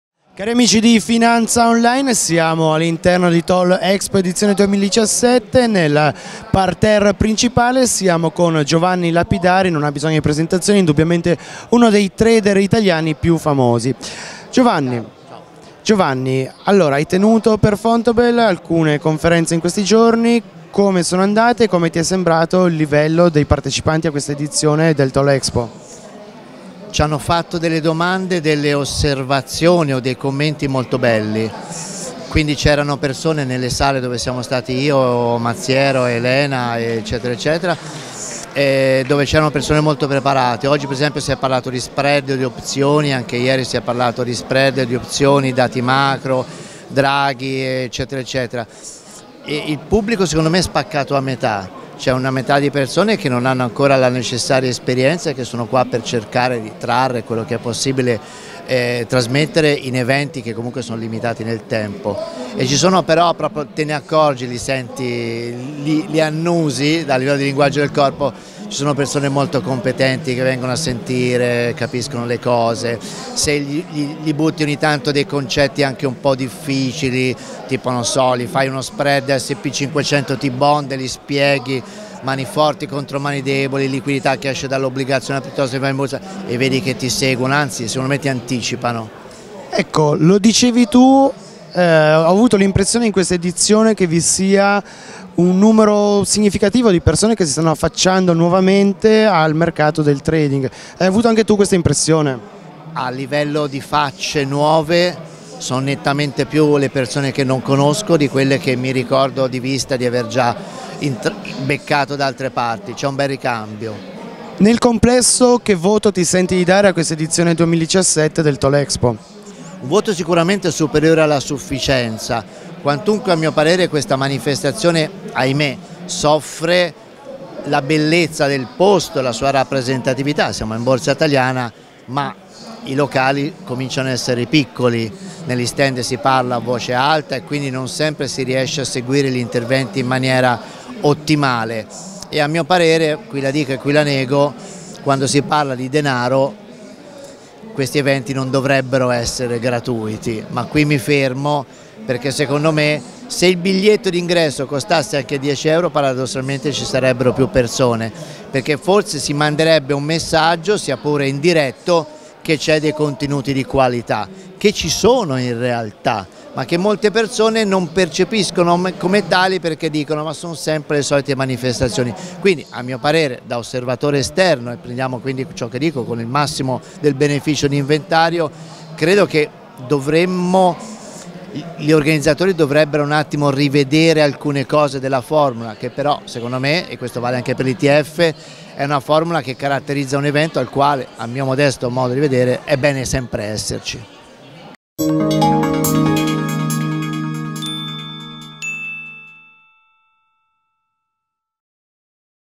TOL EXPO 2017: molti volti nuovi tra i partecipanti a seguire l'evento
In occasione dell’edizione 2017 del TOL Expo organizzato da Borsa Italiana presso Palazzo Mezzanotte, FinanzaOnLine TV ha intervistato alcuni dei protagonisti dell’evento. In questo video Giovanni Lapidari, trader e formatore, evidenzia come nel corso della manifestazione siano intervenuti molti volti nuovi tra gli investitori, spesso con un livello di preparazione già elevato.